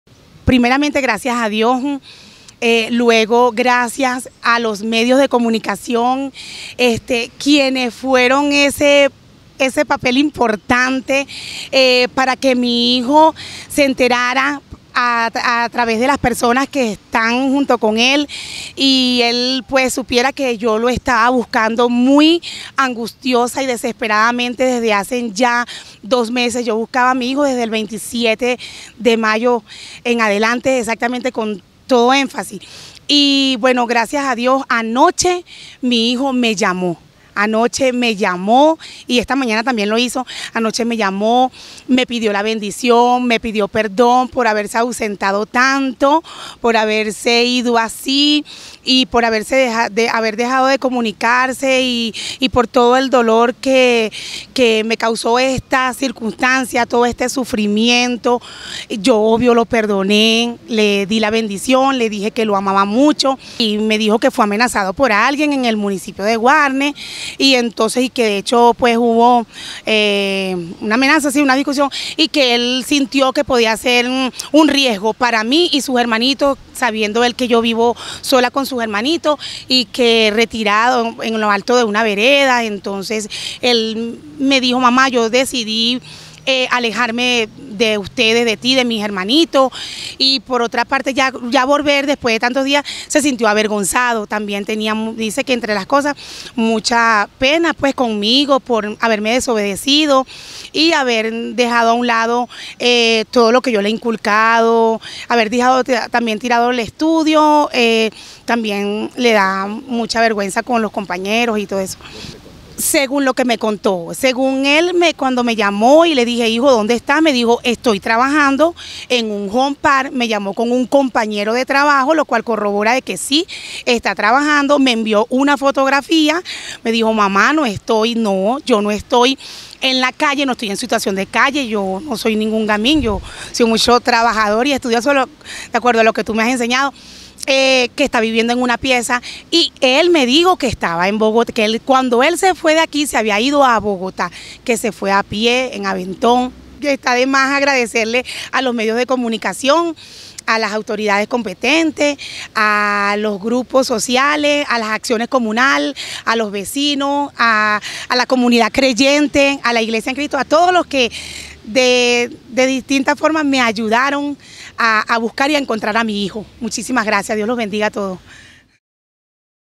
Del llanto a la alegría: el testimonio de Dayanis Pineda tras la aparición de Jesús Gabriel
Desde el 23 de mayo hasta el 10 de julio de 2023, se desconocía el paradero de Jesús Gabriel Mendoza Pineda, el joven a quien su madre lo buscaba desesperadamente en el Oriente antioqueño y en el Área Metropolitana. Según el testimonio de Dayanis Pineda, el menor le habría indicado que por cuestiones de seguridad debió abandonar el municipio de Guarne, que se encuentra bien y para su tranquilidad le compartió esta fotografía. Posteriormente, la mujer agradeció la gestión de los medios de comunicación, de las autoridades y de las demás personas que en su momento ayudaron a buscar a su hijo.